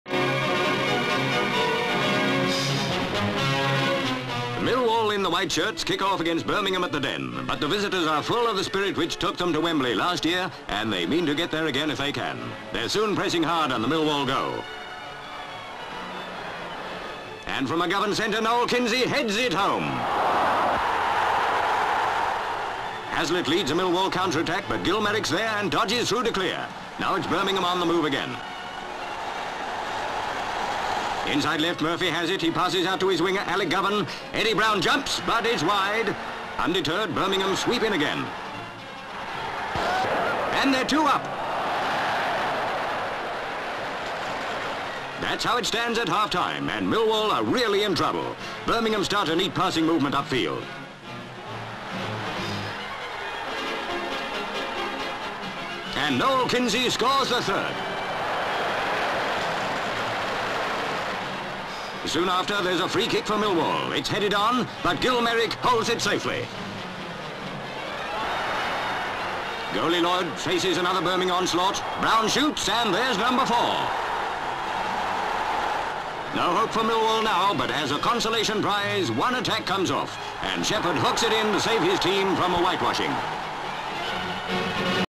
Millwall V Birmingham (1957)
New Cross, London.

Start of the FA (Football Association) Cup 5th round football match between Birmingham City and Millwall. GV. Millwall kick off. David Pacey runs forward with ball but loses it to Eddie Brown. Peter Murphy slips the ball to Noel Kinsey who slips it back to Murphy. He edges onto Brown and Brown starts to run up-field. Birmingham attacks. GV. Massed crowd. LV. Alex Govan throws the ball to team mate who puts it back to him. Govan brings it under control but he is worried by Veitch. He beats him, makes down the left wing and centres. Kinsey heads the ball into net. Birmingham 1- Millwall 0. GV. Crowd. LV. Hazlett crosses the ball into Birmingham goal area where Gill Merrick jumps and grabs it although harassed by a Millwall forward. He turns and clears it up-field where it goes out to the right wing. Birmingham attacking. LV. Lloyd waiting in his goal. SV. Murphy with the ball, slips it out to the left wing. SV. Govan collects and crosses it into the Millwall goal area where Brown makes a leaping dive to head it but it is wide. SV. Crowd. GV. Birmingham on the move. Kinsey with the ball puts a long one into Millwall goal area. SV. Crowd. LV. Ball going into the net and is kicked out by spectator from behind the goal. Birmingham's second goal scored by Govan - we do not see the goal. GV. Massed crowd cheering. Half Time. SV. Right back Jeff Hall passes to Smith who runs on and flicks ball back to Hall who jabs it forward to Watta. Watta lets it go to Gordon Astall who collects and sends a long ball up-field. SV. Crowd. Birmingham's third goal we do not see it again - Noel Kinsey scored this one. LV. Ball is just rolling out of the net, pan to the players congratulating. GV. Massed crowd, part cheering with rattles. LV. Milwall takes a free kick, camera pans. It is headed on but Merrick collects safely and kicks up-field. LV. Lloyd waiting in his goal. LV. Ball comes into the Millwall goal area and Birmingham player kicks it. LV. Ball is rolling out of the goal with Lloyd flat on his face on the ground - Birmingham are four up - again, we did not se the goal. GV. Cheering crowds. SV. Pulley sends it across Birmingham goal area where it beats Merrick. Birmingham defender trying to head it clear. He heads it back to Shepherd who hooks it into the goal for Millwall's consolation. Ball in net. Final score Birmingham City 4, Millwall 1.

(Orig.Neg.)
 FILM ID:633.23